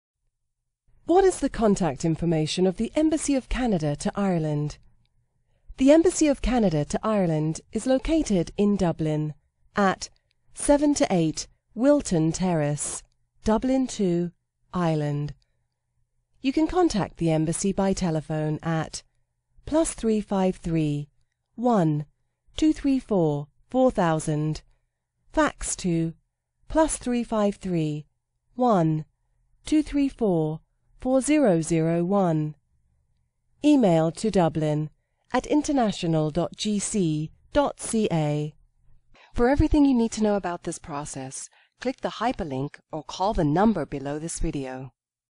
What is the contact information of the Embassy of Canada to Ireland?
The Embassy of Canada to Ireland is located in Dublin at:
• 7-8 Wilton terrace
Dublin 2
Ireland
You can contact the Embassy by:
• telephone, at (+ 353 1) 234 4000
• fax, to (+ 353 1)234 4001